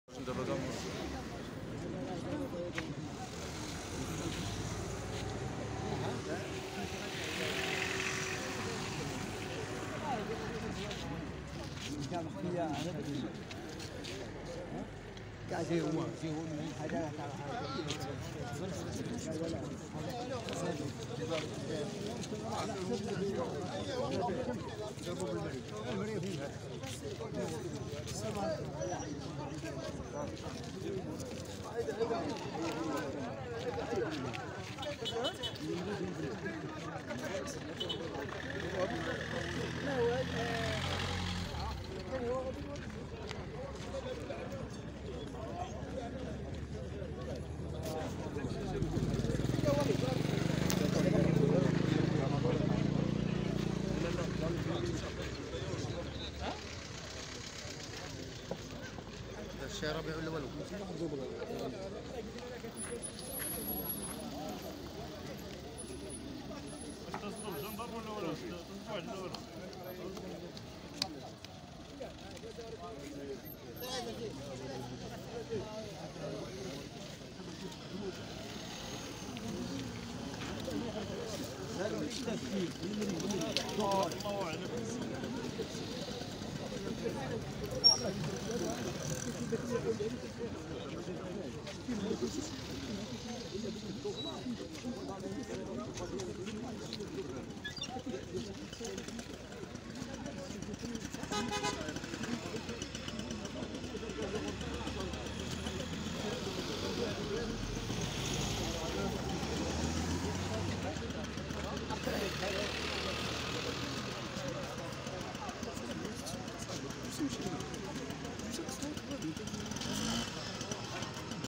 صور حادثة السير التي وقعت عصر اليوم بإدلسان
وقعت قبل قليل حادثة سير نقل على اثرها سيد في سن متقدمة الى المستشفى لاصابته بجروح طفيفة ، وحسب رواية شهود عيان فإن السيد كان راكبا على دراجة هوائية دخل الى المسار المعاكس قادما من الجهة اليمنى دون الانتباه الى السيارة القادمة في غفلة منه مما ادى الى اصطداهم بمرآة سيارة كانت قادمة من اتجاه سكورة . و لحسن حظه لم يصب اصابة تدعو للقلق و جرى نقله لمستشفى سيدي حساين